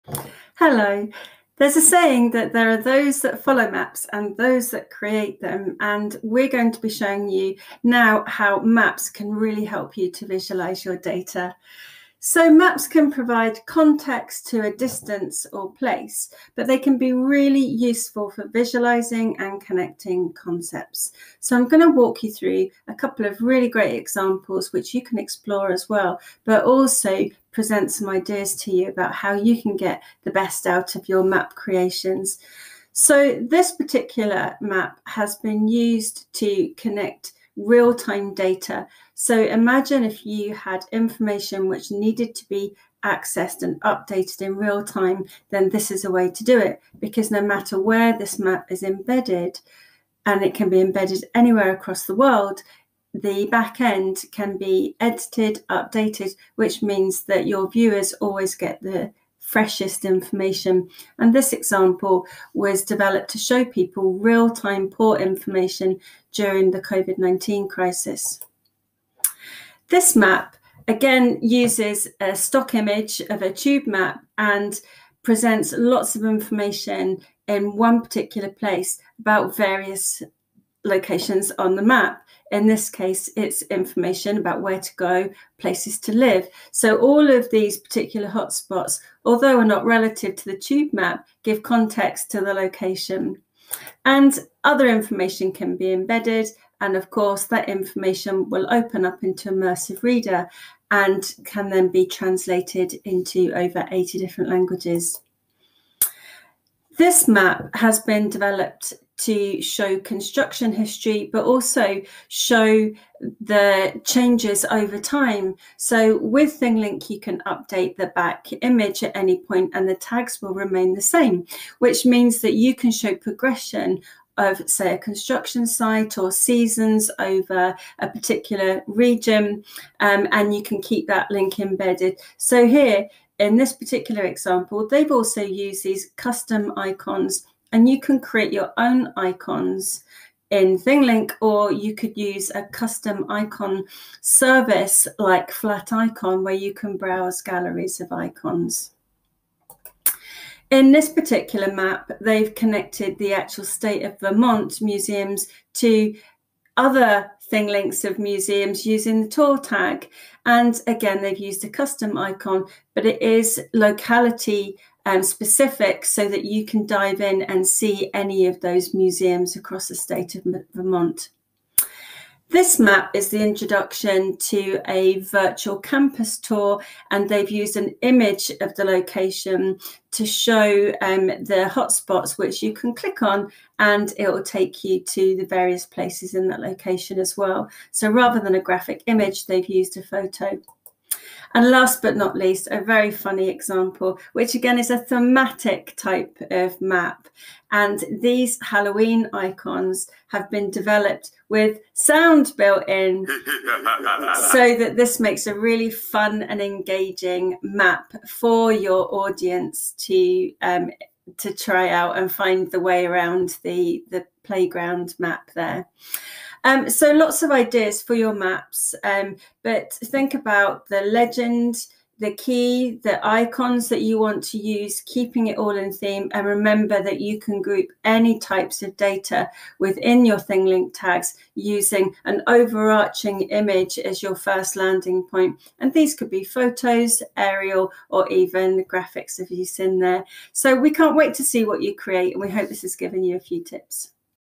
How to create interactive maps and aerial views in ThingLink
ThingLink makes it easy for content creators to design interactive maps and aerial views that boost engagement. Here's a quick how-to create a map or aerial view using the editor.

Give it a try!